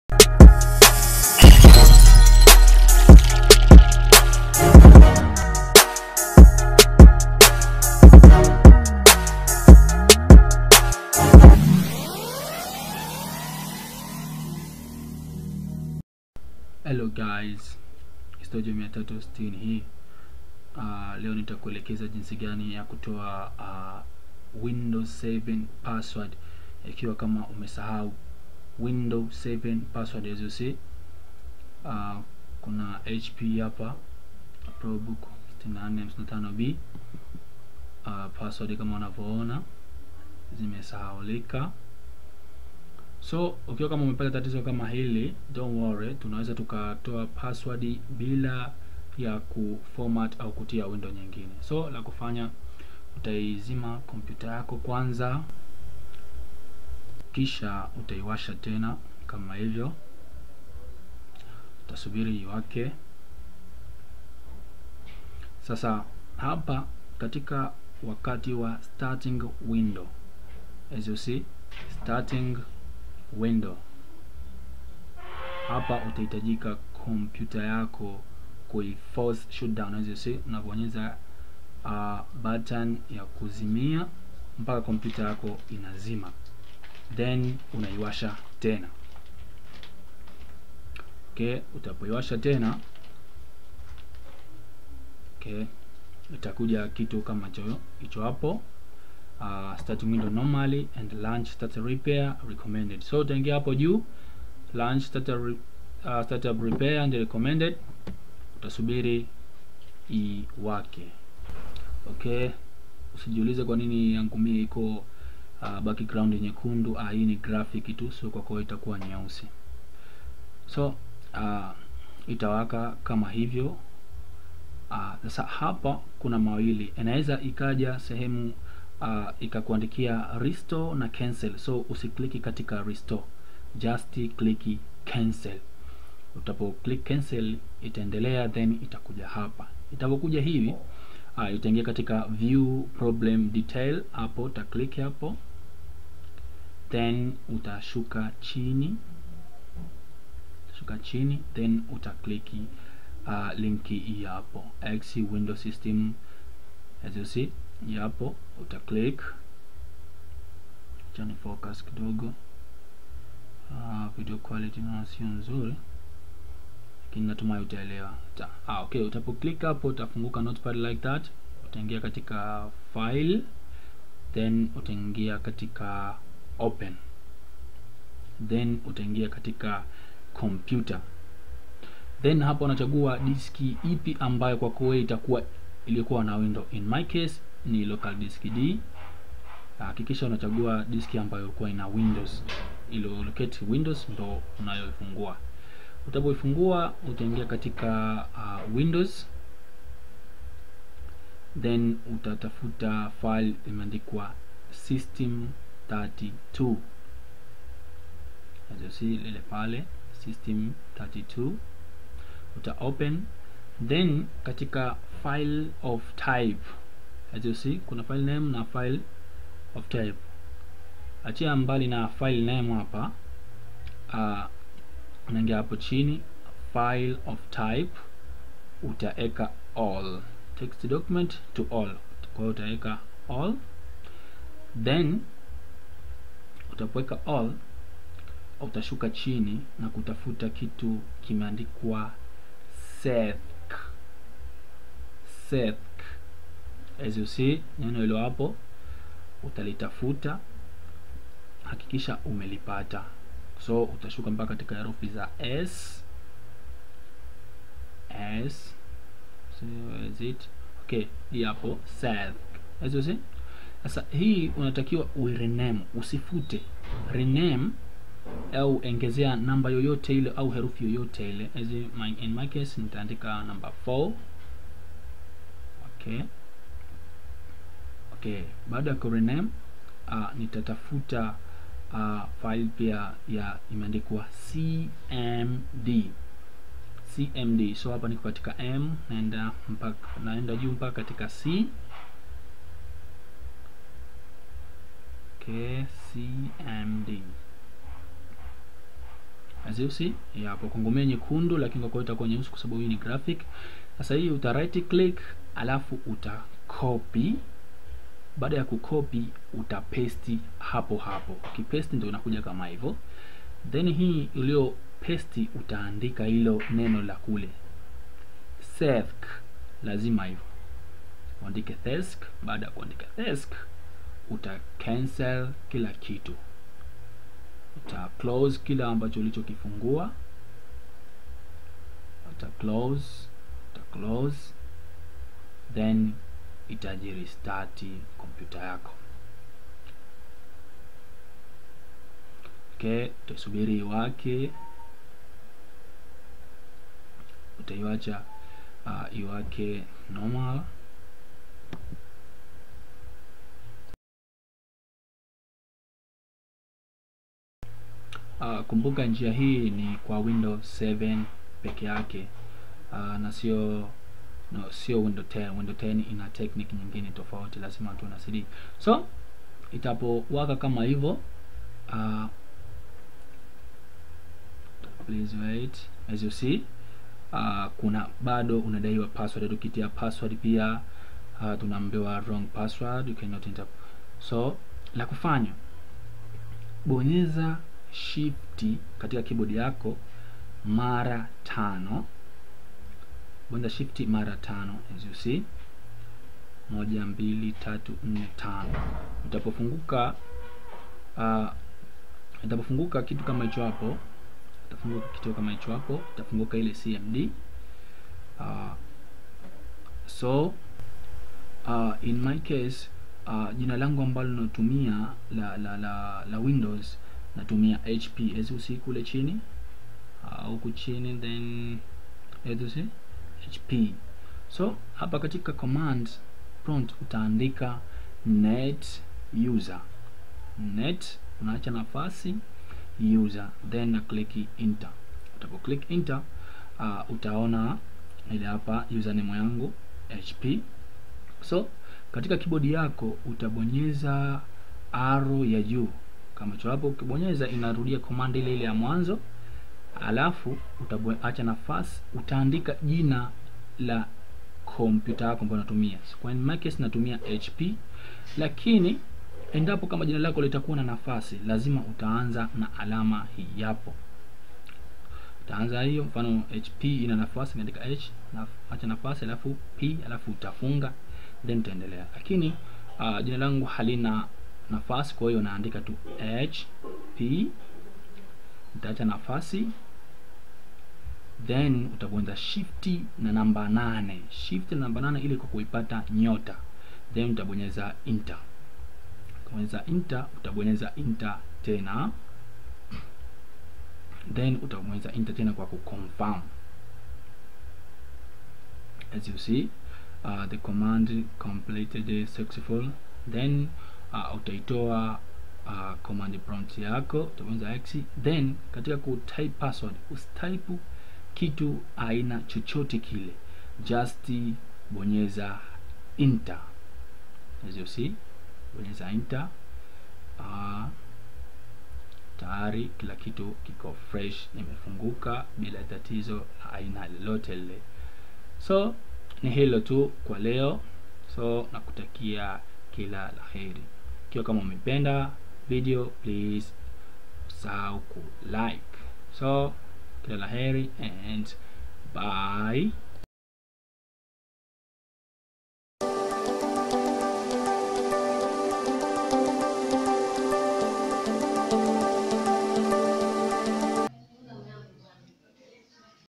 JINSI YA KUTOA WINDOWS 7 PASSWORD
Hi!
SASA UNAWEZA KUTOA PASSWORD KATIKA WINDOWS 7 BILA YA KUTIA WINDOWS AU KUFORMAT HARD DISK.
TOJO 360 BYE.